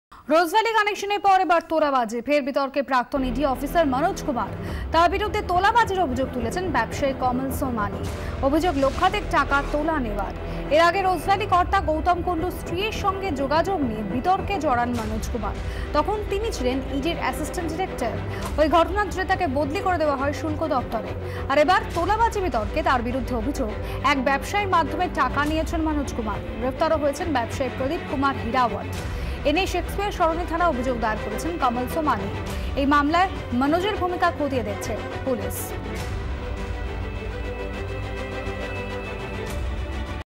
West Bengal: Ex-ED official Manoj Kumar booked in extortion case
West Bengal: Ex-ED official Manoj Kumar booked in extortion case. To satisfy the demands of high-rise favourites of Bengali audiences, Zee 24 Ghanta brings all the latest headlines and news stories.